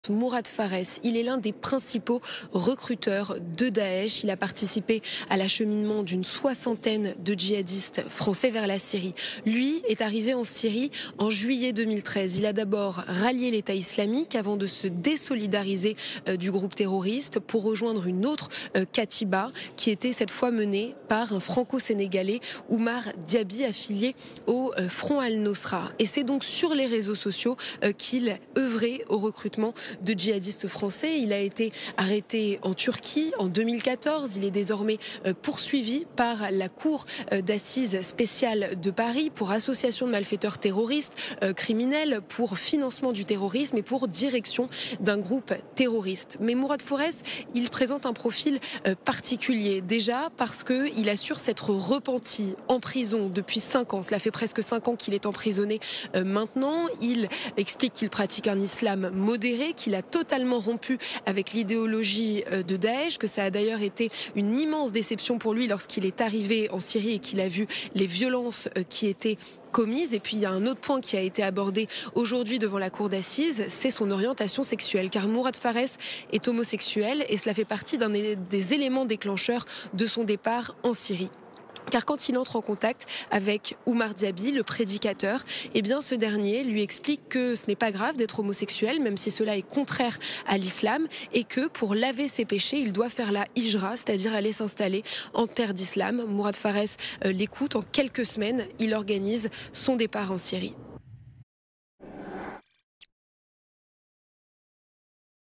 Un présumé recruteur de jihadistes face à la justice
Le procès de Mourad Farès, ancienne figure du recrutement pour le jihad en Syrie et également accusé d'y avoir dirigé un groupe de jeunes combattants francophones, s'est ouvert lundi devant la cour d'assises spéciale de Paris.
Cheveux bruns coupés courts et fine barbe, lunettes, cet homme de 35 ans comparaît seul dans le box, notamment pour association de malfaiteurs terroristes, financement du terrorisme et direction d'un groupe terroriste.
Ces dernières années, le nom de ce transfuge de l'Etat islamique est apparu dans plusieurs dossiers de filières d'acheminement vers la Syrie jugés en correctionnelle à Paris, dont celui du départ de dix jeunes Strasbourgeois en décembre 2013.
Les juges lui imputent l'organisation jusqu'à la mi-2013 de plusieurs réunions évoquant des départs en Syrie, à Paris, Strasbourg ou Lyon.
Il était lui-même parti en juillet 2013, traversant l'Europe en voiture avec trois comparses, après avoir reçu plusieurs virements bancaires.
A son arrivée en Syrie, il avait rejoint les rangs de l'organisation terroriste Etat islamique en Irak et au Levant (EIIL, devenue Etat islamique en 2014), et suivi une formation sur le maniement des armes. Il nie avoir combattu, limitant son rôle à quelques surveillances.
Fin 2013, après s'être désolidarisé de l'EIIL, il avait intégré le groupe du Franco-Sénégalais Oumar Diaby, autre important recruteur de jihadistes toujours visé par un mandat d'arrêt international.
Mourad Farès est soupçonné d'avoir dirigé cette brigade de combattants francophones en l'absence d'Oumar Diaby jusqu'en mars 2014, mais il conteste avoir eu un rôle d'"émir".